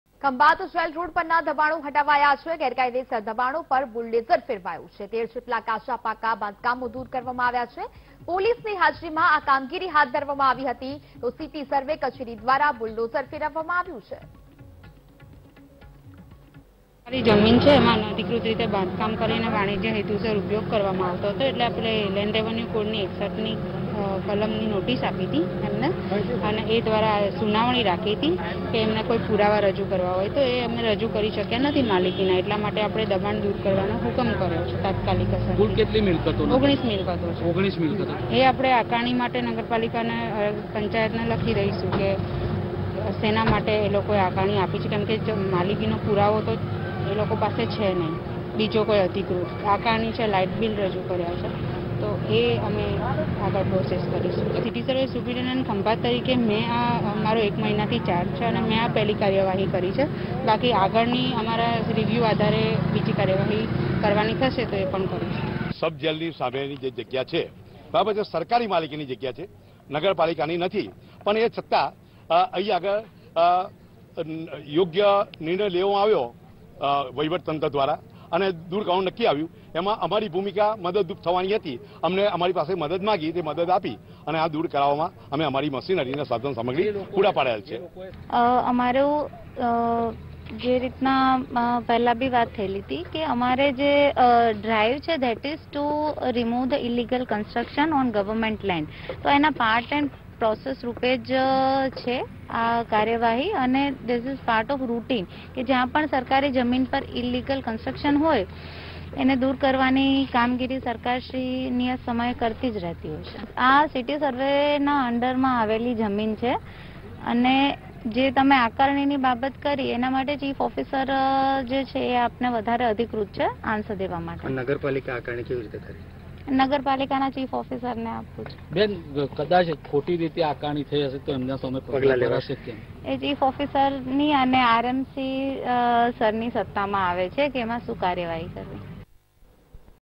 Khambhat માં 13 જેટલા દબાણો પર ફર્યું બુલડોઝર, શોભાયાત્રા પર પથ્થરમારાની ઘટના બાદ તંત્ર સતર્ક
Khambhat માં 13 જેટલા દબાણો પર ફર્યું બુલડોઝર, શોભાયાત્રા પર પથ્થરમારાની ઘટના બાદ તંત્ર સતર્ક  | VTV GUJARATI